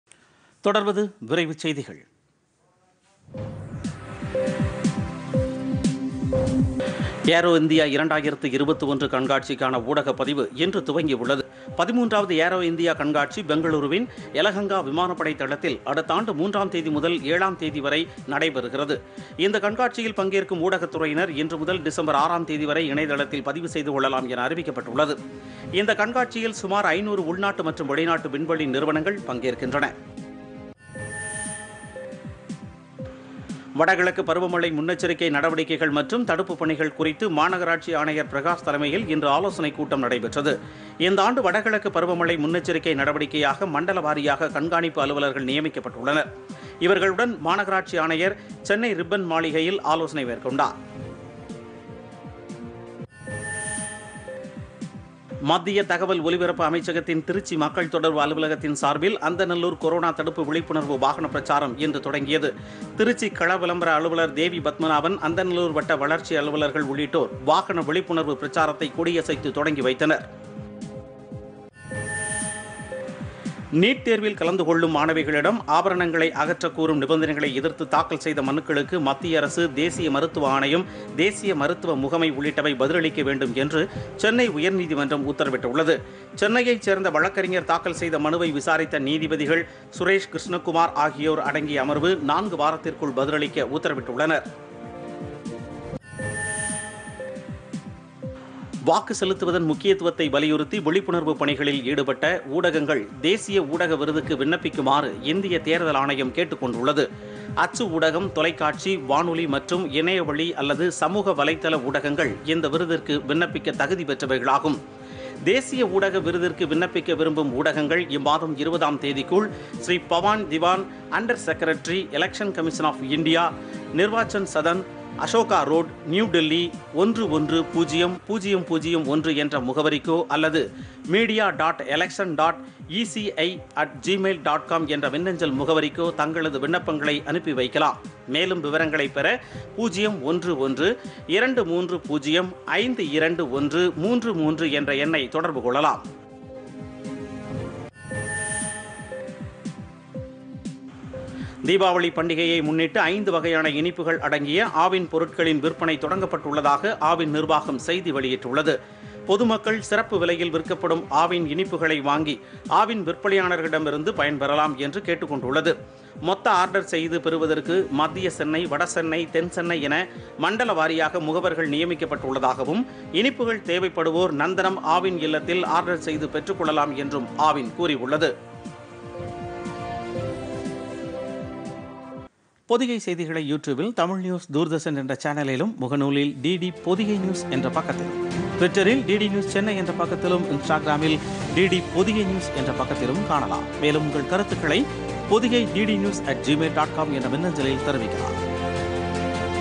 இரவு 7.00 மணி பொதிகை விரைவு செய்திகள் [02.11.2020]
1) ஏரோ இந்தியா 2021 கண்காட்சிக்கான ஊடகப் பதிவு இன்று துவங்கியுள்ளது.  13-வது ஏரோ இந்தியா கண்காட்சி பெங்களூருவின் எலஹங்கா விமானப்படை தளத்தல் அடுத்த ஆண்டு மூன்றாம் தேதி முதல் ஏழாம் தேதி வரை நடைப‍ெறுகிறது.  இந்த கண்காட்சியில் பங்கேற்கும் ஊடகத்துறையினர் இன்று முதல் டிசம்பர் ஆறாம் தேதி வரை இணையதளத்தில் பதிவு செய்து கொள்ளலாம் என  அறிவிக்கப்பட்டுள்ளது.
இந்தக் கண்காட்சியில் சுமார் 500 உள்நாட்டு மற்றும் வெளிநாட்டு விண்வெளி நிறுவனங்கள் பங்கேற்கின்றன,

2) வடகிழக்கு பருவமழை முன்னெச்சரிக்கை நடவடிக்கைகள் மற்றும் தடுப்புப் பணிகள் குறித்து மாநகராட்சி ஆணையர் பிரகாஷ் தலைமையில் இன்று ஆலோசனைக் கூட்டம் நடைபெற்றது.  இந்த ஆண்டு வடகிழக்கு பருவமழை முன்னெச்சரிக்கை நடவடிக்கையாக மண்டல வாரியாக கண்காணிப்பு அலுவலர்கள் நியமிக்கப்பட்டுள்ளனர்.  இவர்களுடன் மாநகராட்சி ஆணையர் சென்னை ரிப்பன் மாளிகையில் ஆலோசனை மேற்கொண்டார்.

3) மத்திய தகவல் ஒலிபரப்பு அமைச்சகத்தின் திருச்சி மக்கள் தொடர்பு அலுவலகத்தின் சார்பில் அந்தாநல்லூரில் ‍கொரோனா தடுப்பு விழிப்புணர்வு வாகன பிரச்சாரம் இன்று தொடங்கியது.  திருச்சி கள விளம்பர அலுவலர் தேவி பத்மநாபன் அந்தநல்லூர் வட்ட வளர்ச்சி அலுவலர்கள் உள்ளிட்டோர் வாகன விழிப்புணர்வு பிரச்சாரத்தை கொடியசைத்து  தொடங்கி வைத்தனர்.

4)  நீட்தேர்வில்  கலந்துகொள்ளும் மாணவிகளிடம் ஆபரணங்களை அகற்றக்கூறும் நிபந்தனைகளை எதிர்த்து தாக்கல் செய்த மனுக்களுக்கு மத்திய அரசு, தேசிய மருத்துவ ஆணையம், தேசிய மருத்துவ முகமை உள்ளிட்டவை பதிலளிக்க வேண்டும் என்று சென்னை உயர்நீதிமன்றம் உத்தரவிட்டுள்ளது.  சென்னையை சேர்ந்த வழக்கறிஞர் தாக்கல் செய்த மனுவை விசாரித்த நீதிபதிகள் சுரேஷ், கிருஷ்ணகுமார் ஆகியோர் அடங்கிய அமர்வு நான்கு வாரத்திற்குள் பதிலளிக்க உத்தரவிட்டுள்ளனர்.

5)   வாக்கு செலுத்துவதின் முக்கியத்துவத்தை வலியுறுத்தி விழிப்புணர்வு பணிகளில் ஈடுபட்ட ஊடகங்கள் தேசிய ஊடக விருதுக்கு விண்ணப்பிக்குமாறு இந்திய தேர்தல் ஆணையம் கேட்டுக்‍கொண்டுள்ளது. அச்சு ஊடகம், தொலைக்காட்சி, வானொலி மற்றும் இணையவழி அல்லது சமூக வலைதள  ஊடகங்கள் இந்த விருதுகக்கு விண்ணப்பிக்க தகுதி பெற்றவைகளாகும்.  தேசிய ஊடக விருதுக்கு விண்ணப்பிக்க விரும்பும் ஊடகங்கள் இம்மாதம் 20 ஆம் தேதிக்குள் Sri Pawan Diwan, Under Secretary (Commiunication), Election Commission of India, Nirvachan Sadan, Ashoka Road, New Delhi - 110 001 என்ற முகவரிக்கோ அல்லது என்ற மின்னஞ்சல் முகவரிக்கோ தங்களது விண்ணப்பங்களை அனுப்பி வைக்கலாம். மேலும் விவரங்களை ‍பெற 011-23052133 என்ற எண்ணை தொடர்பு கொள்ளலாம்.

6)  தீபாவளி பண்டிகையை முன்னிட்டு ஐந்து வகையான இனிப்புகள் அடங்கிய ஆவின் பொருட்களின் விற்பனை தொடங்கப்பட்டுள்ளதாக ஆவின் நிர்வாகம் செய்தி வெளியிட்டுள்ளது. பொதுமக்கள் சிறப்பு விலையில் விற்கப்படும் ஆவின் இனிப்பு வகைகள‍ை ஆவின் விற்பனையாளர்களிடமிருந்து வாங்கி பயன்பெறலாம் என்று கேட்டுக்கொண்டுள்ளது. மொத்தமாக ஆடர் செய்து பெறுவதற்கு மத்திய சென்னை, வடசென்னை, தென் சென்னை என மண்டல வாரியாக  முகவர்கள் நியமிக்கப்பட்டுள்ளதாகவும் இனிப்புகள் தேவைப்படுவோர் நந்தனம்  ஆவின் இல்லத்தில் ஆர்டர் செய்து பெற்றுக்கொள்ளலாம் என்றும் ஆவின் கூறியுள்ளது.